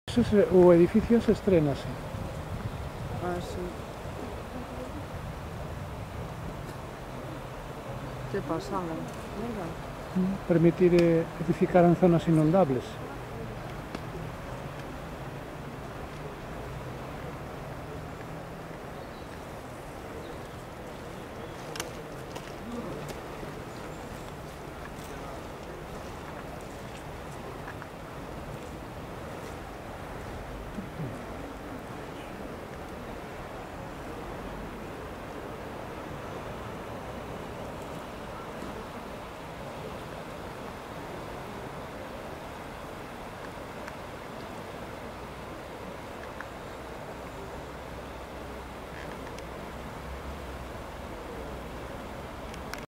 RIADA RIO UMIA EN CALDAS DE REIS 06-01-2011
As periódicas riadas do rio umia co desbordamento do rio provocan a forza e arrastres da auga o se paso pola vila de Caldas; calquer obstaculo na zona anegable supon un risco para a seguridade